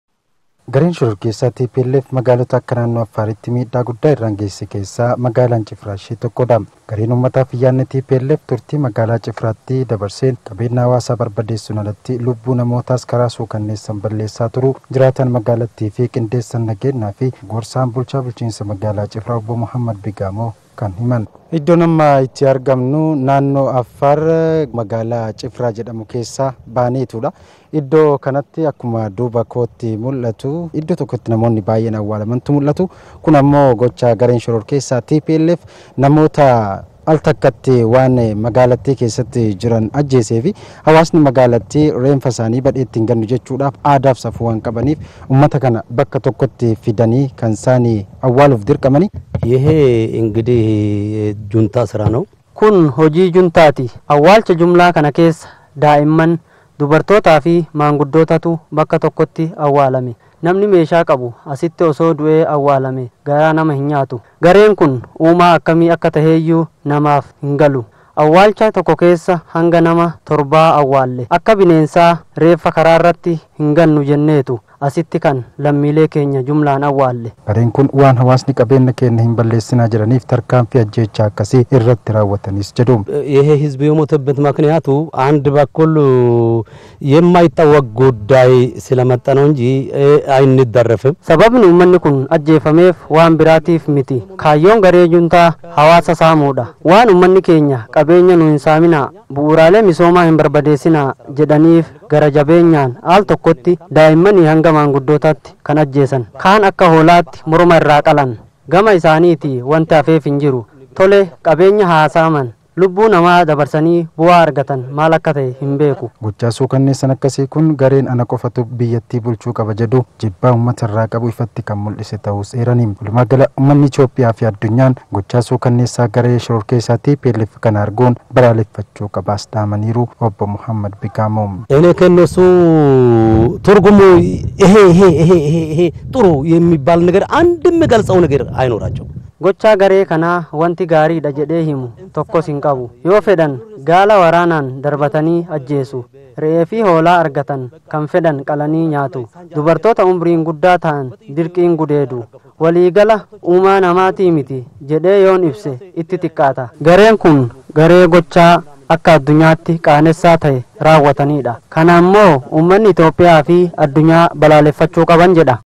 Gareen shororkeessa TPLF ajjeechaa Jumilaa magaala Cifiraatti raawwachuu isa himame.
Gareen shororkeessa TPLF ajjeechaa Jumilaa magaala Cifiraatti raawwachuu isaJirattoonni Magaalitti himan.